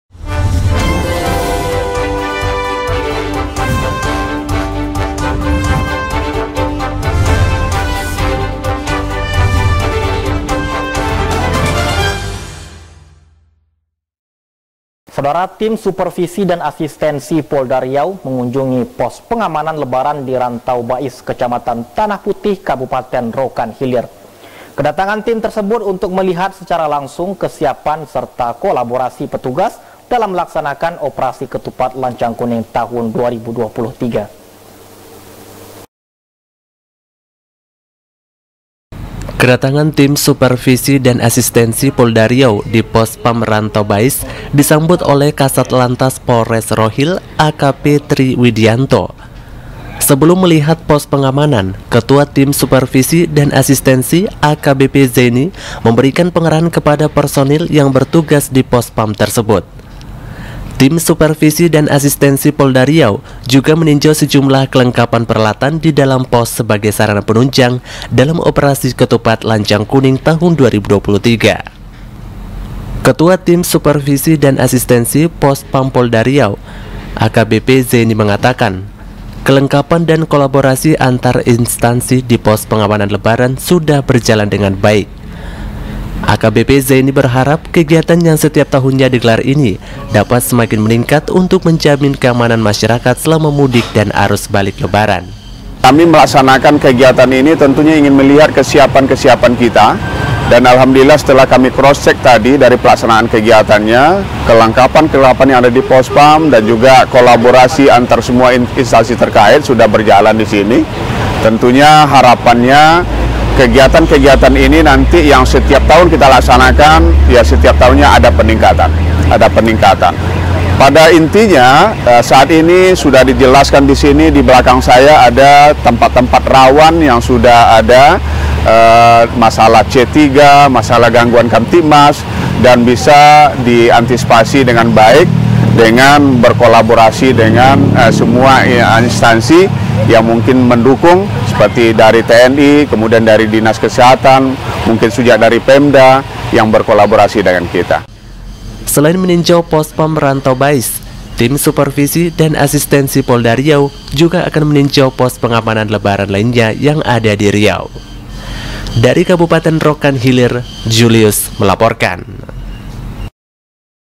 POLDA RIAU SUPERVISI DAN ASISTENSI KUNJUNGI POSPAM RANTAU BAIS ROHIL
ROHIL (riautelevisi.com) - Tim Supervisi dan Asistensi Polda Riau mengunjungi pos pengamanan lebaran di Rantau Bais Kecamatan Tanah Putih Kabupaten Rokan Hilir. Kedatangan tim tersebut untuk melihat secara langsung kesiapan serta kolaborasi petugas dalam melaksanakan Operasi Ketupat Lancang Kuning Tahun 2023.

Kedatangan Tim Supervisi dan Asistensi Polda Riau di Pospam Rantau Bais disambut oleh Kasatlantas Polres Rohil AKP Tri Widiyanto. Sebelum melihat pos pengamanan, Ketua Tim Supervisi dan Asistensi AKBP Zaini memberikan pengarahan kepada personil yang bertugas di Pospam tersebut. Tim Supervisi dan Asistensi Polda Riau juga meninjau sejumlah kelengkapan peralatan didalam pos sebagai sarana penunjang dalam operasi ketupat lancang kuning tahun 2023. Ketua Tim Supervisi dan Asistensi Pospam Polda Riau AKBP Zaini mengatakan, kelengkapan dan kolaborasi antar instansi di pos pengamanan lebaran sudah berjalan dengan baik, AKBP Zaini berharap kegiatan yang setiap tahunnya di gelar ini dapat semakin meningkat untuk menjamin keamanan masyarakat selama mudik dan arus balik lebaran.

Selain meninjau Pospam Rantau Bais, Tim Supervisi dan Asistensi Polda Riau juga akan meninjau pos pengamanan lebaran lainnya yang ada diriau. (Julius)